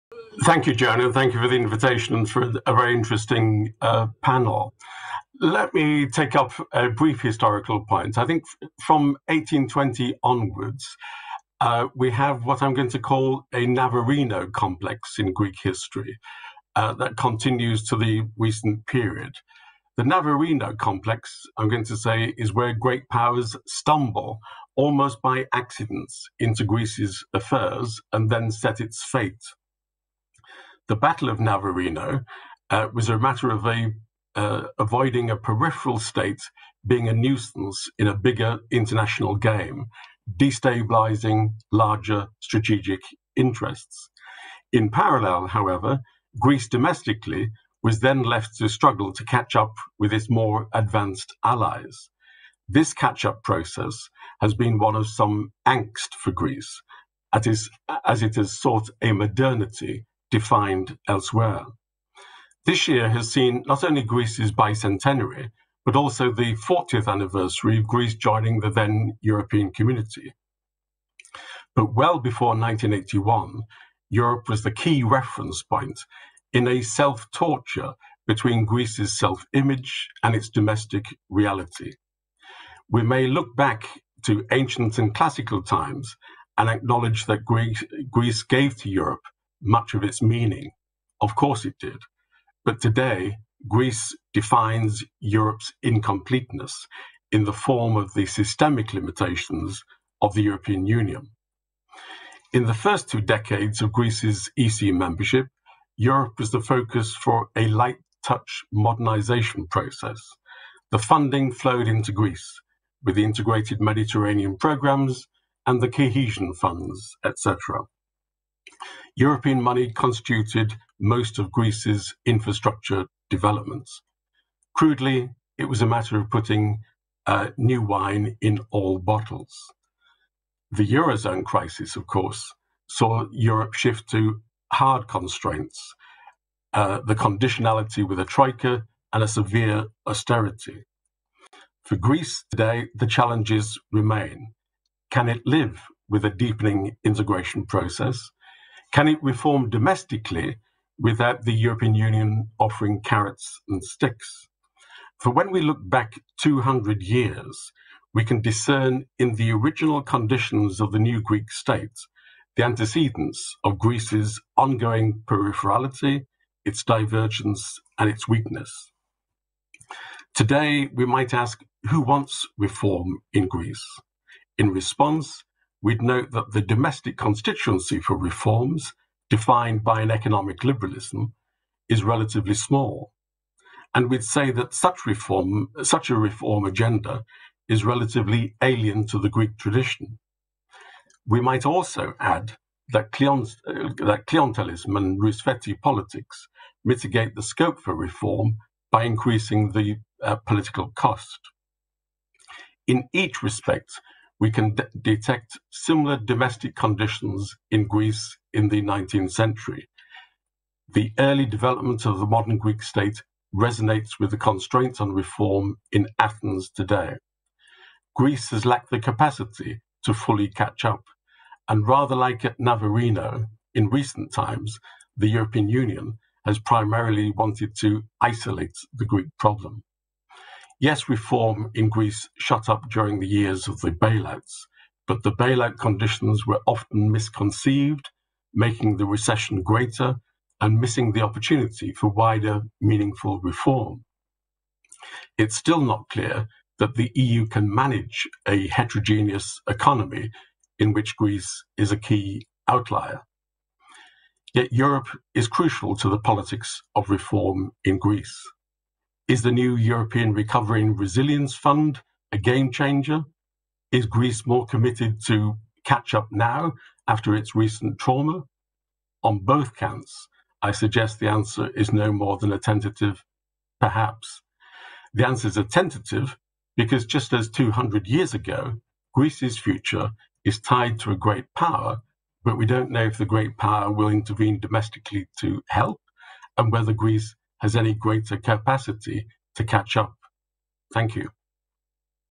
Kevin Featherstone on 200 years of contemporary Greece, challenges and achievements
Kevin Featherstone, Eleftherios Venizelos professor in contemporary Greek studies, professor in European politics in the European institute, LSE